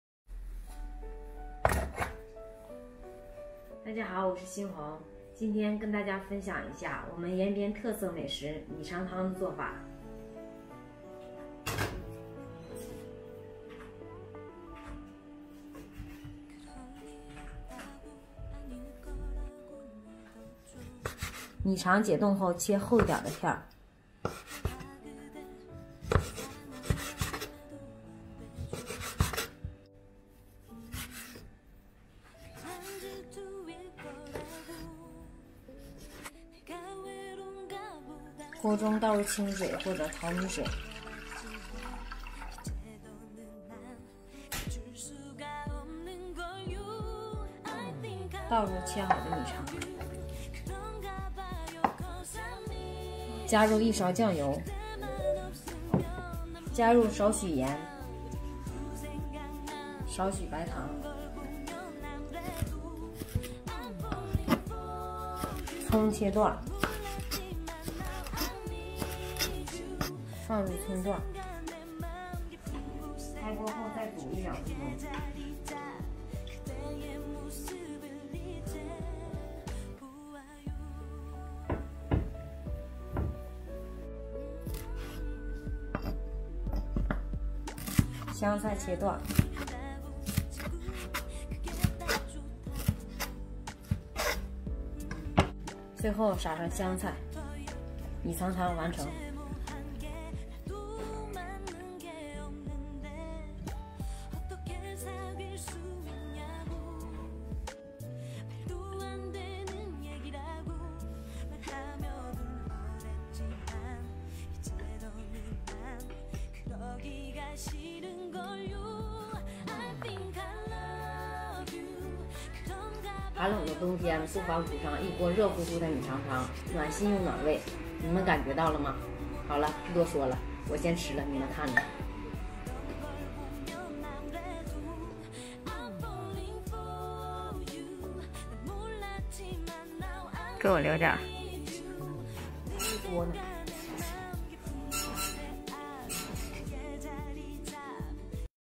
延边朝鲜族特色米肠汤咋做？冒着热气腾腾 煮上一锅吃胃暖暖的！
【新红与小新】延边朝鲜族特色米肠汤咋做？冒着热气腾腾 煮上一锅吃胃暖暖的！

寒冷冬季煮上一锅米肠汤，延边朝鲜的特色，冒着热气腾腾真诱人！

东北大小姐：分享东北一家人的点滴生活